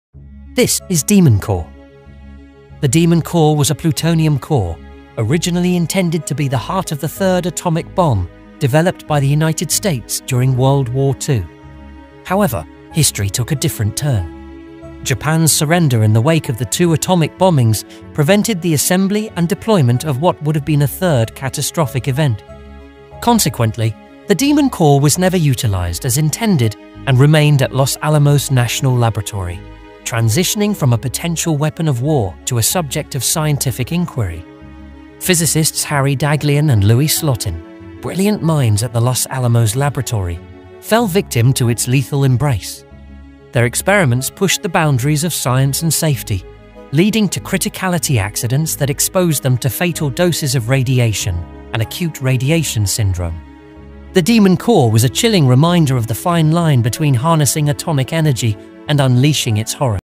The Demon Core ☢️|IQFactory
"The Demon Core" is a gripping documentary exploring the harrowing events surrounding one of the deadliest nuclear accidents in history. Join us as we delve into the true story of the Demon Core, a plutonium core at the heart of two catastrophic incidents that claimed the lives of brilliant scientists. Through compelling archival footage and expert interviews, witness the race against time and the devastating consequences of pushing scientific boundaries. Discover the untold human stories behind this tragic chapter in nuclear history and its enduring impact on science and safety protocols. Uncover the mysteries of the Demon Core in this captivating and thought-provoking documentary.